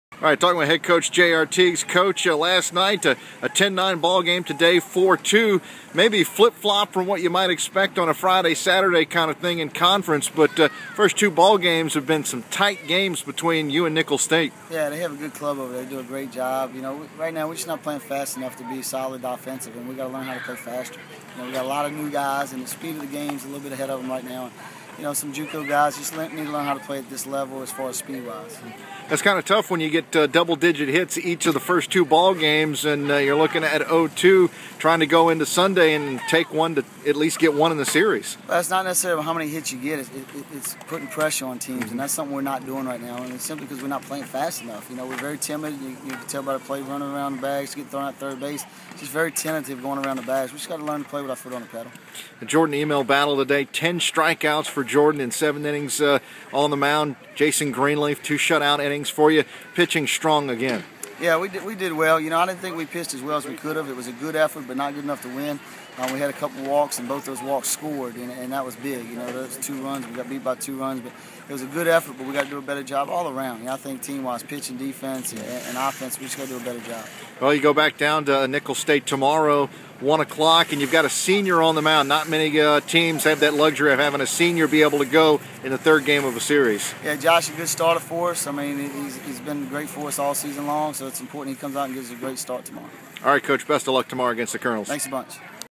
Jay Artigues postgame
Lions fall to Nicholls State for second game